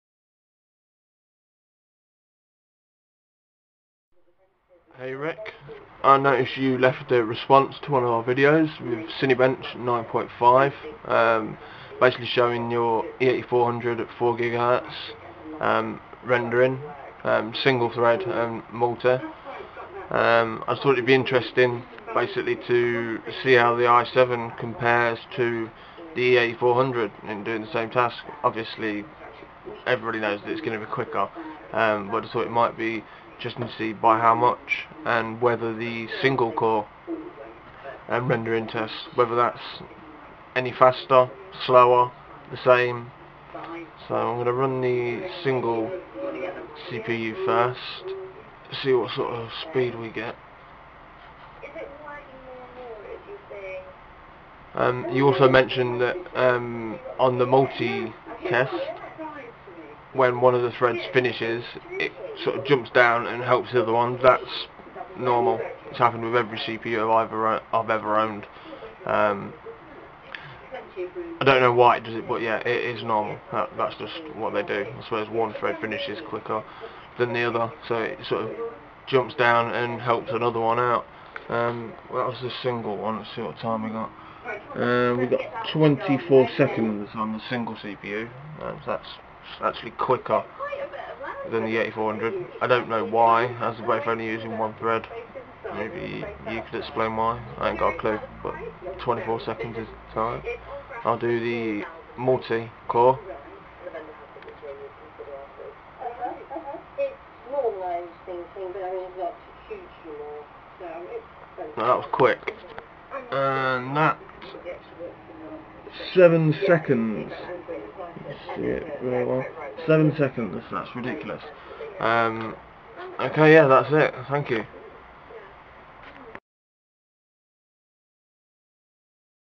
INTEL CORE I7 CINEBENCH 9.5
jUST TO SEE WHAT SORT OF DIFFERENCE IN RENDERING POWER HAS OVER A DUEL CORE CLOCKED AT THE SAME SPEED